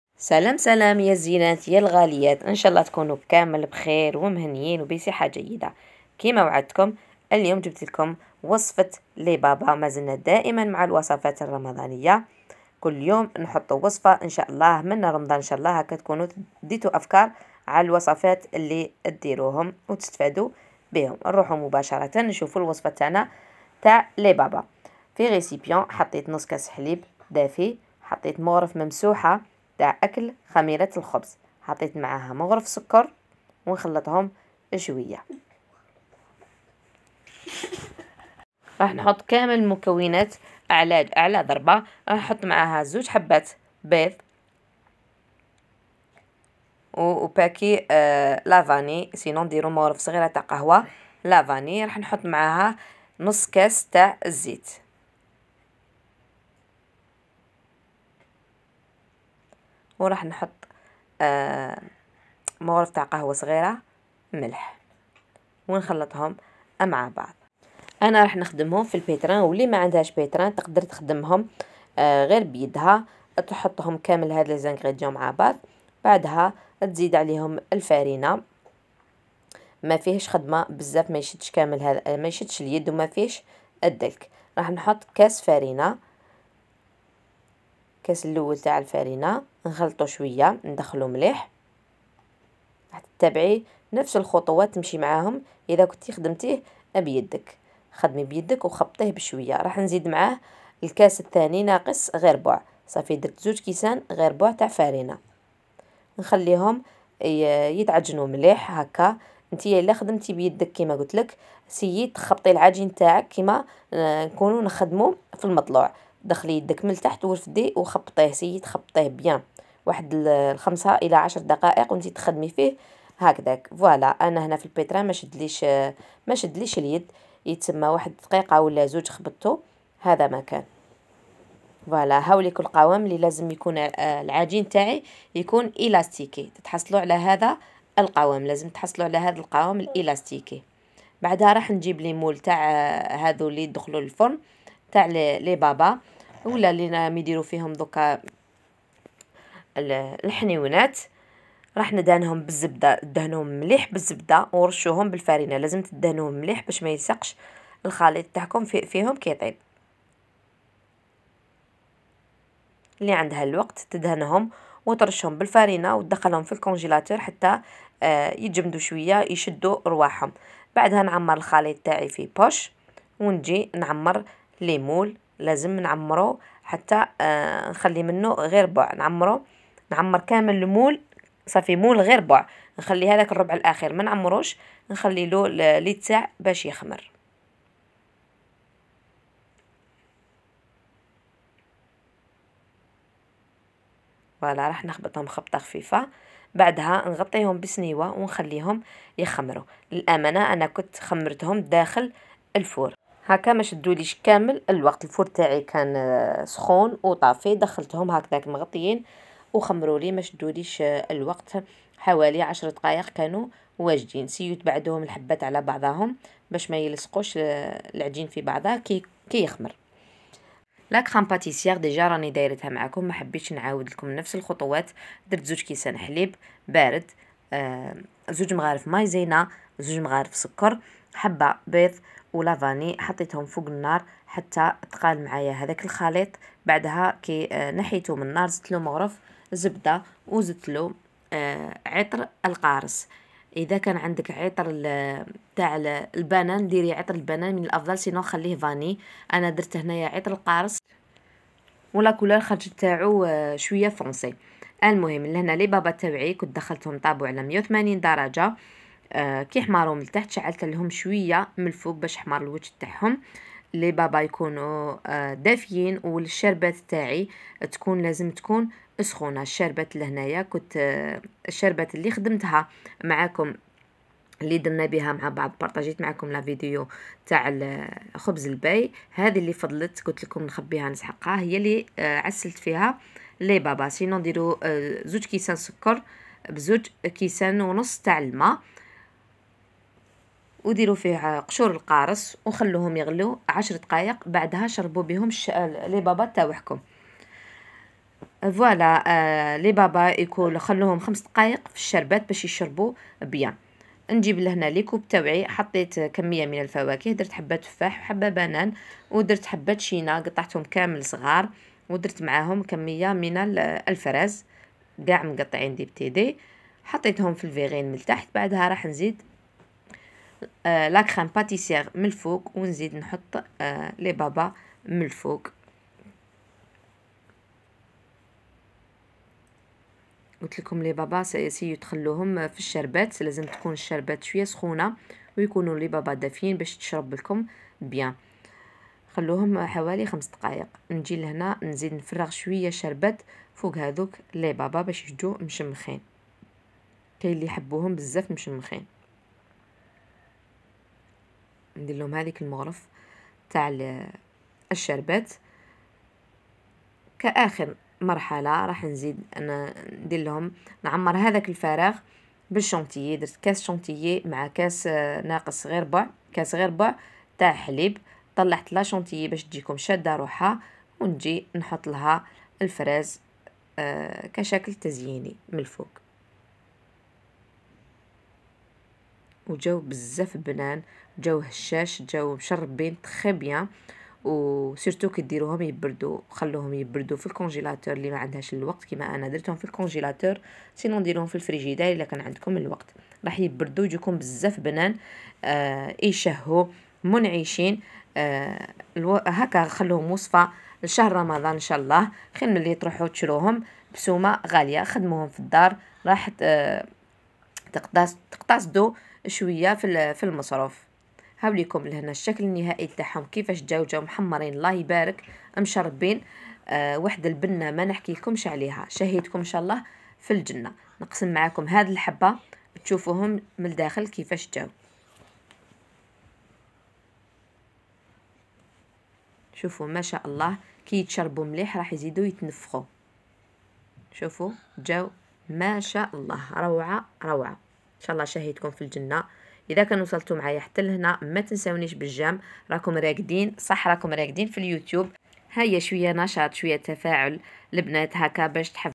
لي بابا خفاف مشربين حتى للقلب وصفات رمضان 2023 يذوبو ذوبان
مقادير لي بابا
نصف كاس حليب
نصف كاس زيت
مغرف ك خميرة الخبز ممسوحة
مغرف ك سكر /
2كيسان غير ربع فرينة
 2بيض

لي -بابا#ام-ايمن-ريم#رمضان-2023